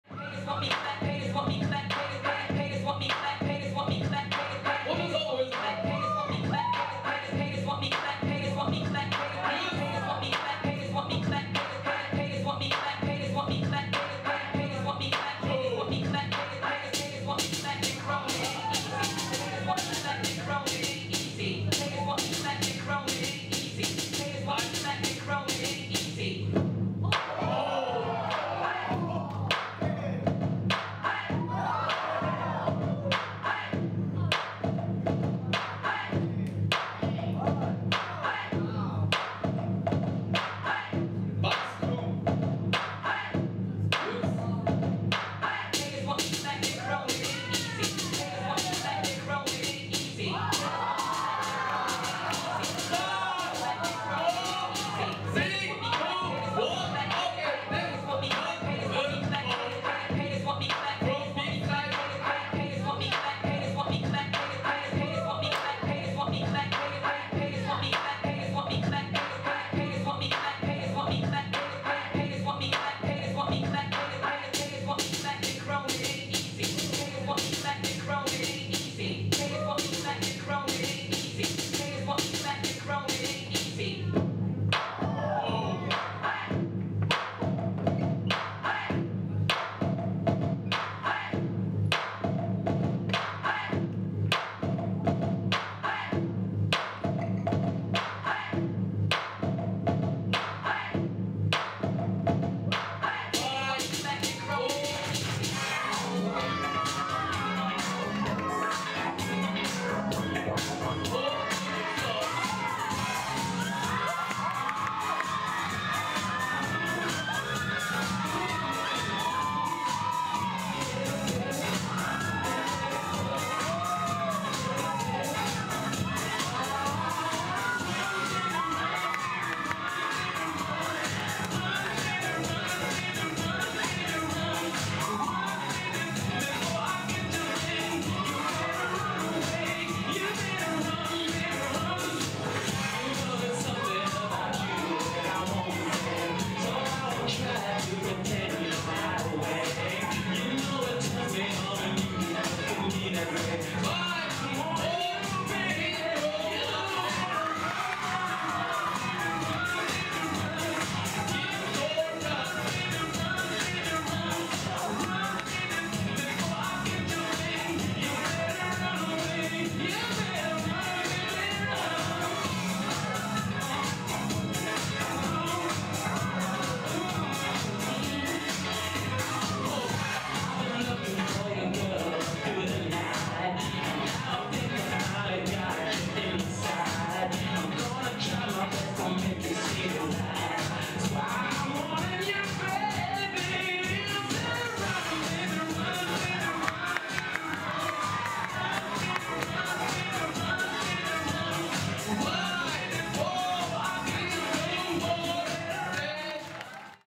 어서오쉐어 vol.2 - 2 on 2 mixed battle Quarter Final 2 Jeem/Yuns VS 김도현/전은호
어서오쉐어 vol.2

2 on 2 MIXED BATTLE Quarter Final 2
(w) Jeem Yuns VS 김도현 전은호

   JUDGE LINE UP -
SEMI ( XEBEC )
EUN.G ( THE B )
WAACKXXXY ( FEELS WAACK )
LUNA YUNA ( LUPLO D-PLO )

(어서오쉐어) - GWANGJU CITY

@MOVIN'UP DANCE STUDIO
by,Bitgoeul Dancers